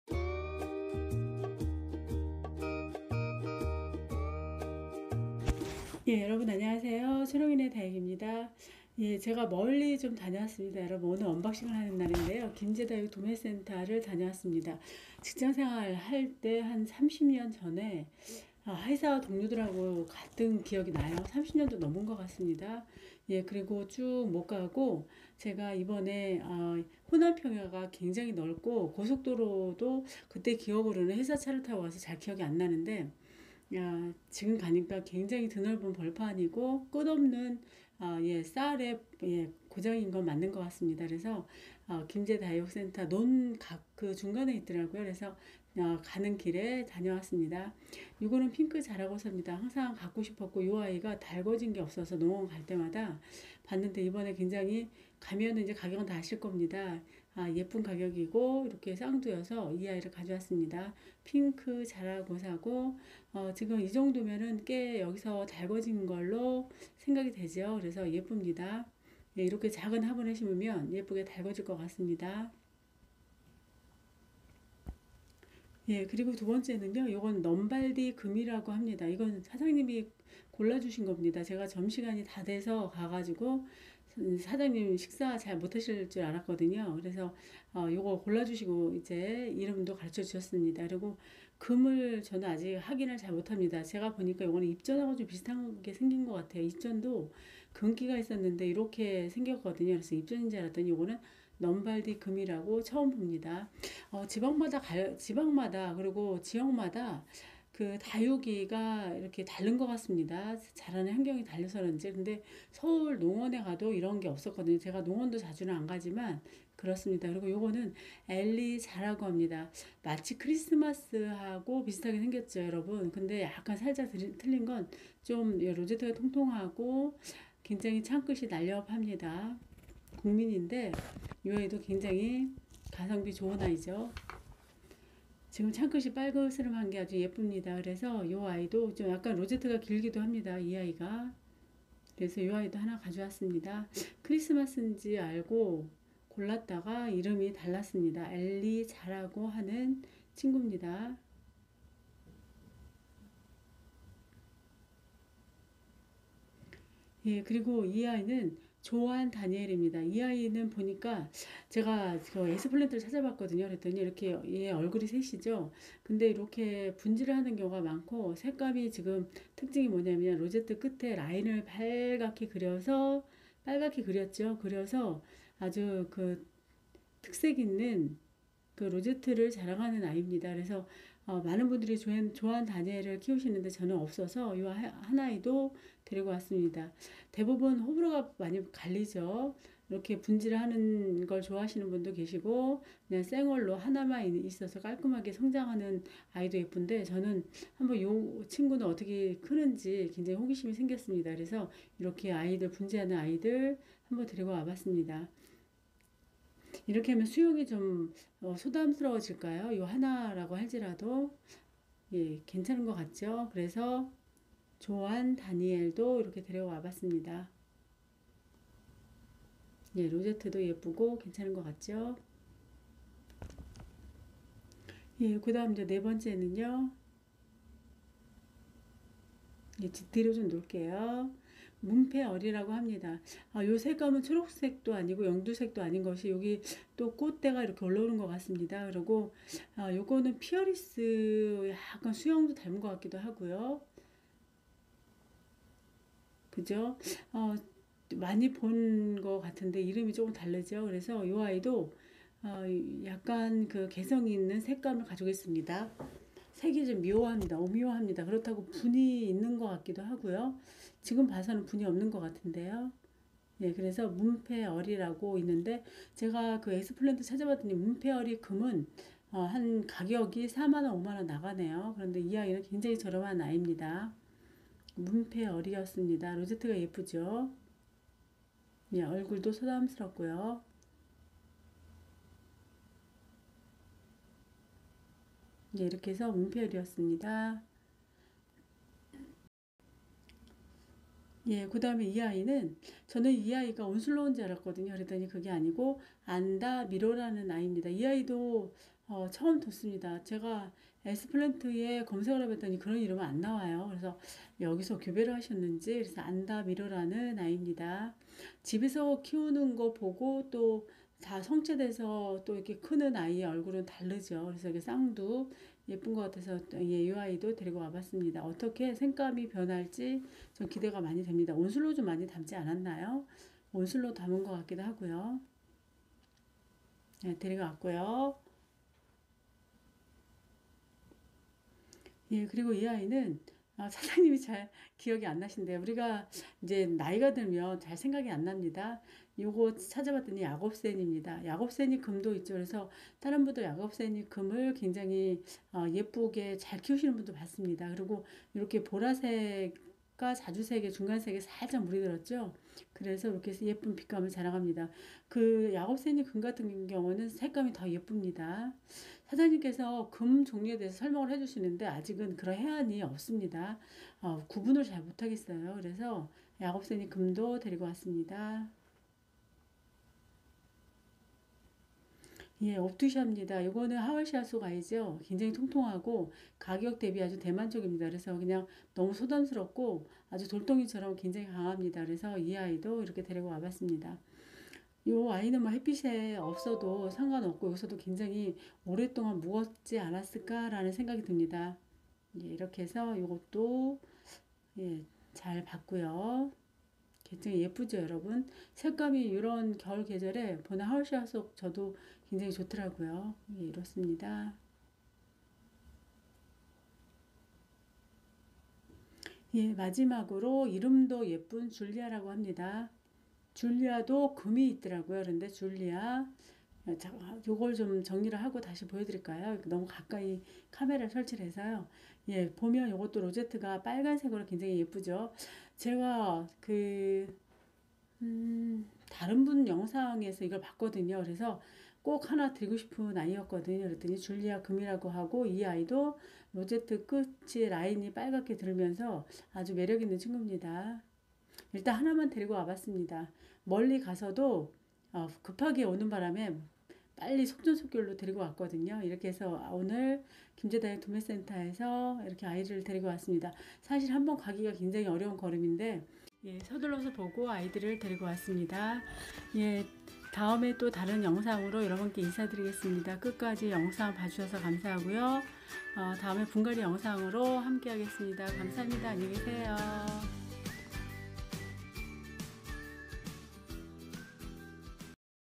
김제다육식물도매센터 다육 생소한 이름 많아요 (런발디금, 엘리자 문페어리 안다미로 줄리아 .... )
전라북도 김제시 금산면 봉은길 101-88 지번 전라북도 김제시 금산면 삼봉리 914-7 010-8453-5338

김제다육식물도매센터는
국민다육부터 창 종류까지 다양한 곳입니다.

제가 다육을 시작하면서 영상으로 접한

 곳으로, 일이 있어서 김제에 갔다가

 들렀습니다.

  처음 가본 곳인데, 호남평야의 드넓은

 들판이 끝없이 펼쳐진 좋은

 곳이었습니다.

센스업 모듈형 폴딩선반 5단세트 (5.3.2 단변신/높이조절/확장/분리) 홈앤쇼핑

겨울냉해피해
다육페트병이 심기